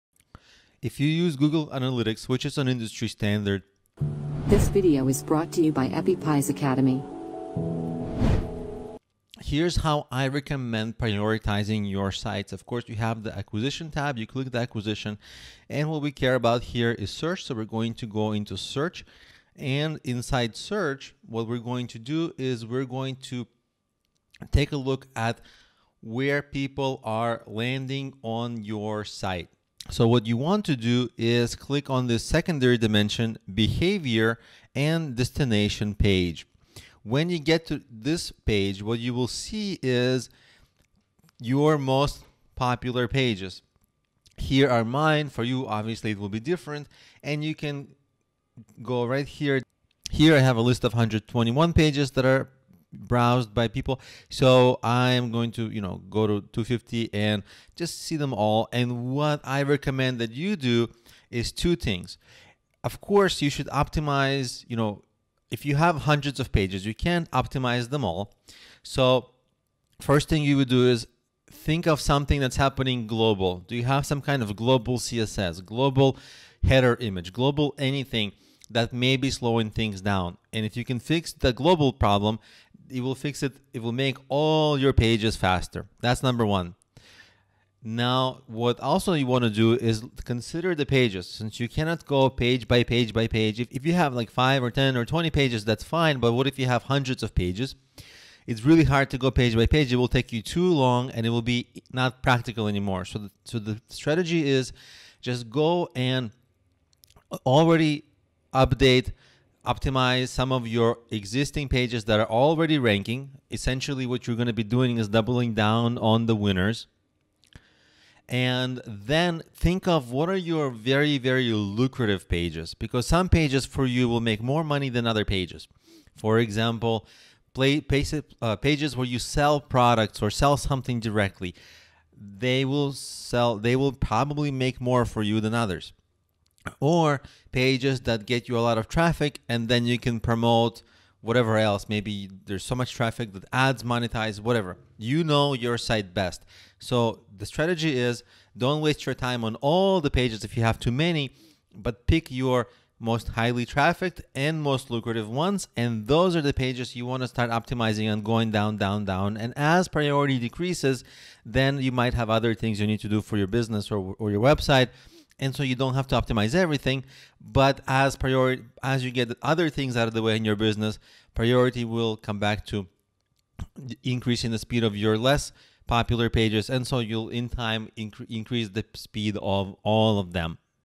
Page optimization strategy: Lesson 23
Page optimization strategy: Lesson 23 - Nothing is more annoying than a slow website. A slow website can affect your traffic, page views, conversions, sales, and the overall reputation of your business. This means that website speed is one of the most critical factors to keep your bottom-line up.
This course from Appy Pie Academy will teach you how to improve your website loading speed in no time. This video tutorial covers some of the most common things that reduce website speed, including lack of text compression, large images, render-blocking resources, and too much JavaScript. Watch this video tutorial to learn some of the most amazing techniques to increase your website speed.

To learn more about "Technical SEO Optimization: How to Increase Website Speed", please